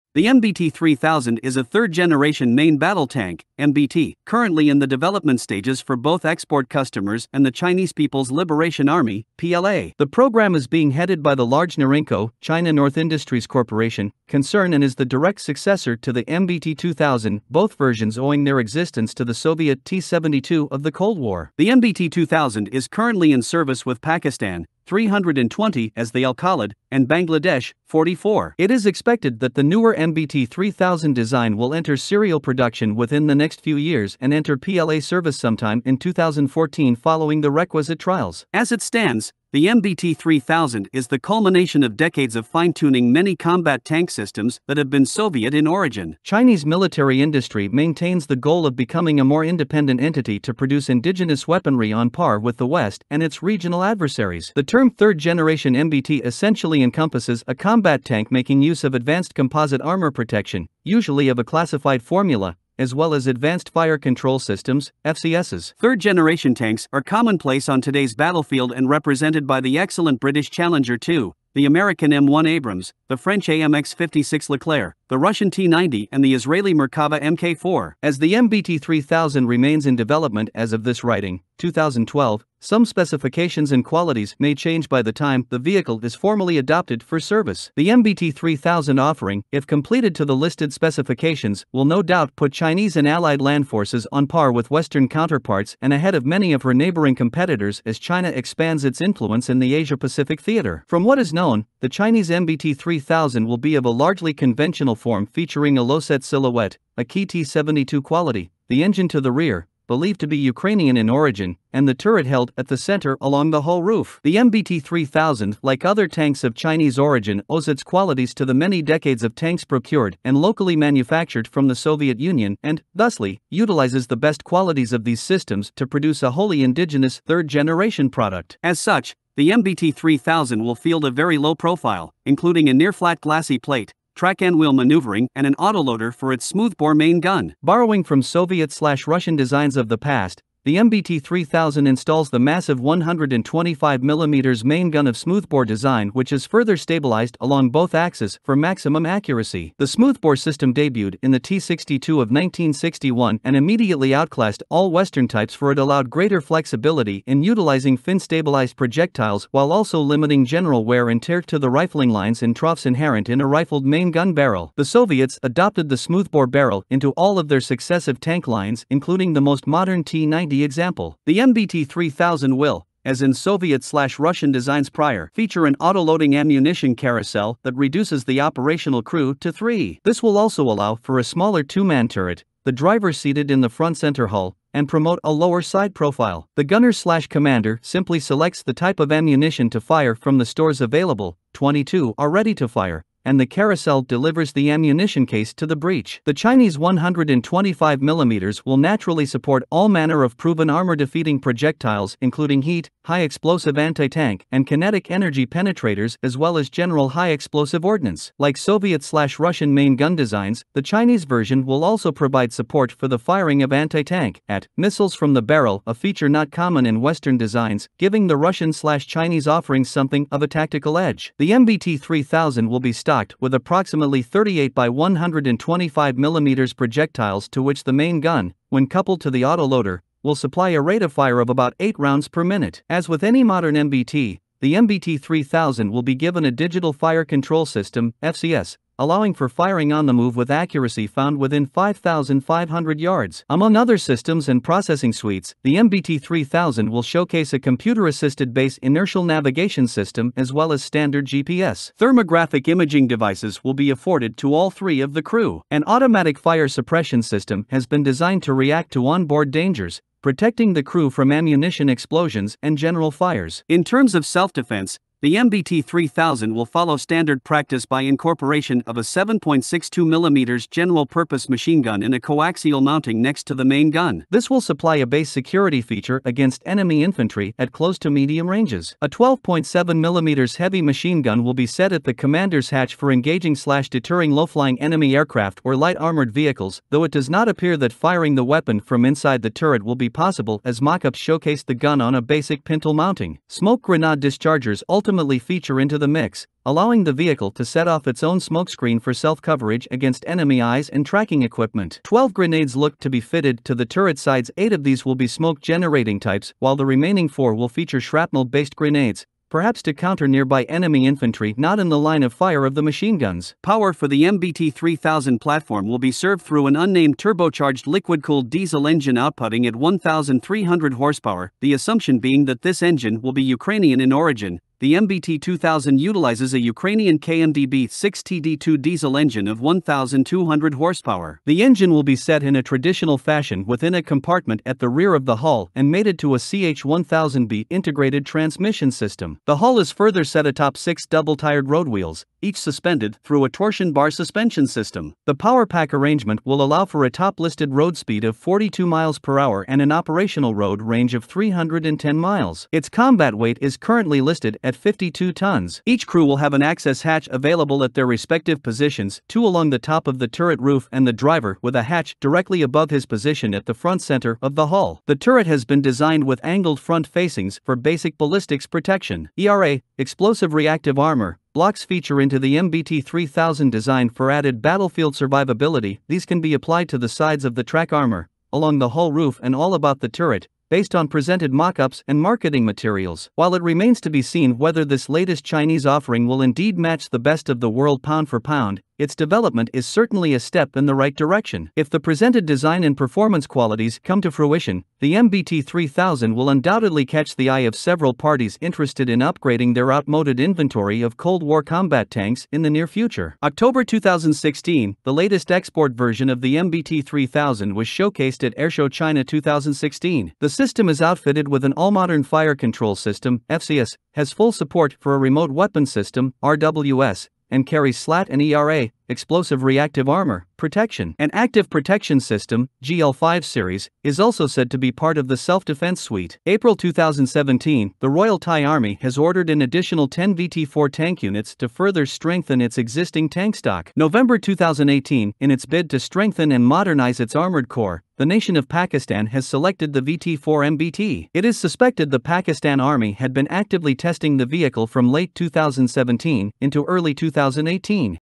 NORINCO VT-4 (MBT-3000)
The MBT-3000 is a 3rd Generation Main Battle Tank (MBT) currently in the development stages for both export customers and the Chinese People's Liberation Army (PLA). The program is being headed by the large NORINCO (China NORth INdustries COrporation) concern and is the direct successor to the "MBT-2000" - both versions owing their existence to the Soviet "T-72" of the Cold War. The MBT-2000 is currently in service with Pakistan (320 as the "Al-Khalid") and Bangladesh (44). It is expected that the newer MBT-3000 design will enter serial production within the next few years and enter PLA service sometime in 2014 following the requisite trials. As it stands, the MBT-3000 is the culmination of decades of fine-tuning many combat tank systems that have been Soviet in origin. Chinese military industry maintains the goal of becoming a more independent entity to produce indigenous weaponry on par with the West and its regional adversaries.

The term "3rd Generation MBT" essentially encompasses a combat tank making use of advanced composite armor protection (usually of a classified formula) as well as advanced fire control systems (FCSs). 3rd Generation Tanks are commonplace on today's battlefield and represented by the excellent British Challenger 2, the American M1 Abrams, the French AMX-56 Leclerc, the Russian T-90 and the Israeli Merkava Mk IV. As the MBT-3000 remains in development as of this writing (2012), some specifications and qualities may change by the time the vehicle is formally adopted for service. The MBT-3000 offering - if completed to the listed specifications - will no doubt put Chinese and allied land forces on par with Western counterparts and ahead of many of her neighboring competitors as China expands its influence in the Asia-Pacific Theater.

From what is known, the Chinese MBT-3000 will be of a largely conventional form featuring a low-set silhouette (a key T-72 quality), the engine to the rear (believed to be Ukrainian in origin) and the turret held at the center along the hull roof. The MBT-3000 - like other tanks of Chinese origin - owes its qualities to the many decades of tanks procured and locally manufactured from the Soviet Union and, thusly, utilizes the best qualities of these systems to produce a wholly indigenous 3rd Generation product. As such, the MBT-3000 will field a very low profile (including a near-flat glacis plate), track-and-wheel maneuvering and an autoloader for its smoothbore main gun.

Borrowing from Soviet/Russian designs of the past, the MBT-3000 installs the massive 125mm main gun of smoothbore design which is further stabilized along both axis for maximum accuracy. The smoothbore system debuted in the T-62 of 1961 and immediately outclassed all Western types for it allowed greater flexibility in utilizing fin-stabilized projectiles while also limiting general wear and tear to the rifling lines and troughs inherent in a "rifled" main gun barrel. The Soviets adopted the smoothbore barrel into all of their successive tank lines including the most modern T-90 example.

The MBT-3000 will, as in Soviet/Russian designs prior, feature an autoloading ammunition carousel that reduces the operational crew to three. This will also allow for a smaller two-man turret (the driver seated in the front-center hull) and promote a lower side profile. The gunner/commander simply selects the type of ammunition to fire from the stores available (22 are ready-to-fire) and the carousel delivers the ammunition case to the breech. The Chinese 125mm will naturally support all manner of proven armor-defeating projectiles including HEAT (High Explosive Anti-Tank) and kinetic energy penetrators as well as general high explosive ordnance. Like Soviet/Russian main gun designs, the Chinese version will also provide support for the firing of anti-tank (AT) missiles from the barrel - a feature not common in Western designs, giving the Russian/Chinese offerings something of a tactical edge. The MBT-3000 will be stocked with approximately 38 x 125mm projectiles to which the main gun, when coupled to the autoloader, will supply a rate-of-fire of about eight rounds per minute.

As with any modern MBT, the MBT-3000 will be given a digital fire-control system (FCS) allowing for firing "on-the-move" with accuracy found within 5,500 yards. Among other systems and processing suites, the MBT-3000 will showcase a computer-assisted base inertial navigation system as well as standard GPS. Thermographic imaging devices will be afforded to all three of the crew. An automatic fire suppression system has been designed to react to onboard dangers, protecting the crew from ammunition explosions and general fires.